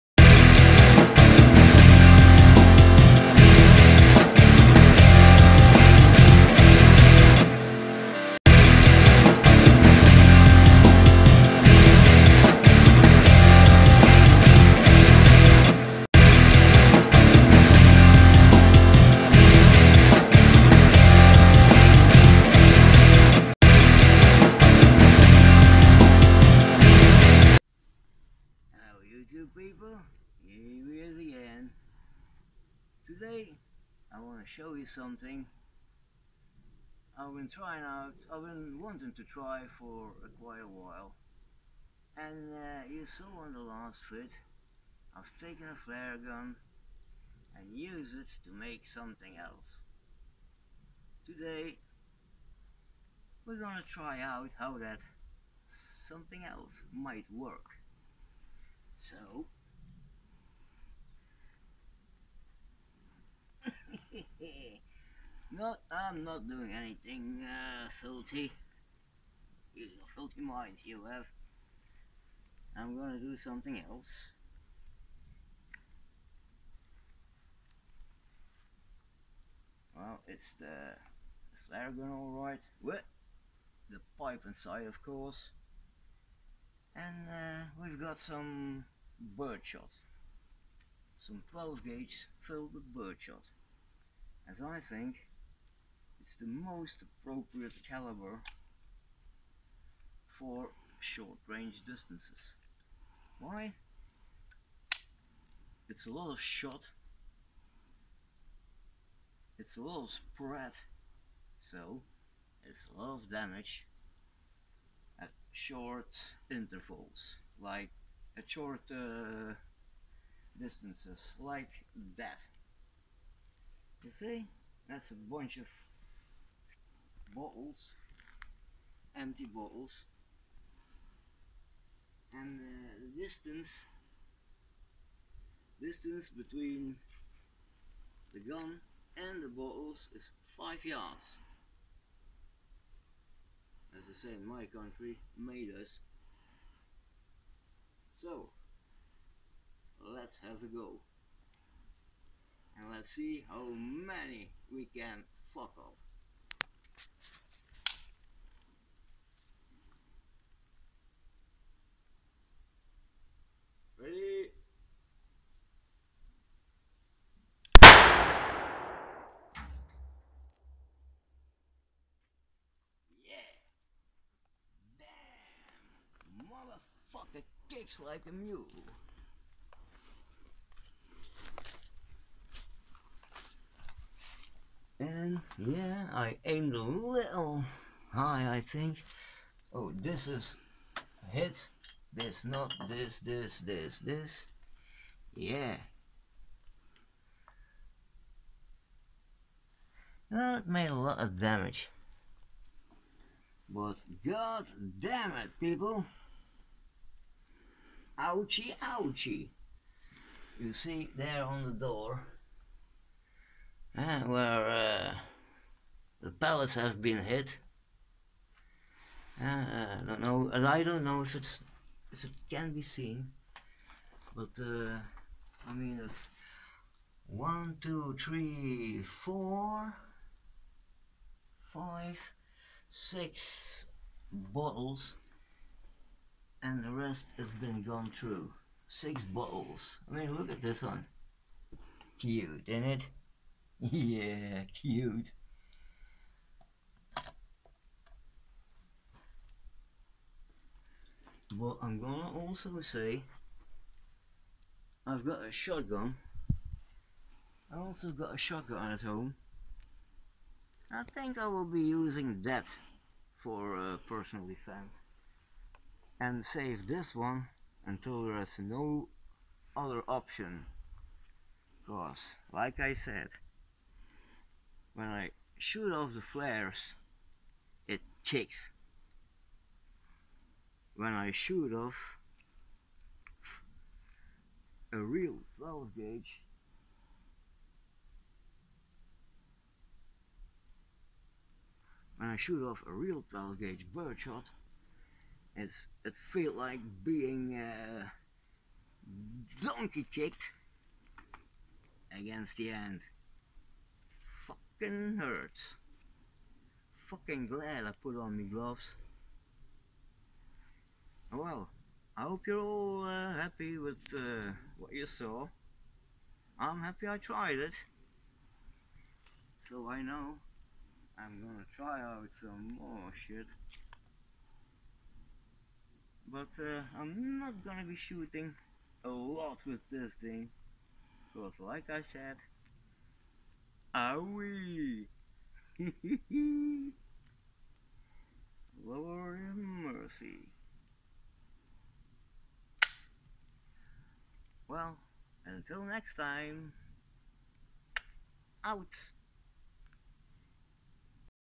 Flaregun 12 gauge birdshot
Shooting a live 12 gauge from a modified flaregun...
GREAT outcome, awful recoil.!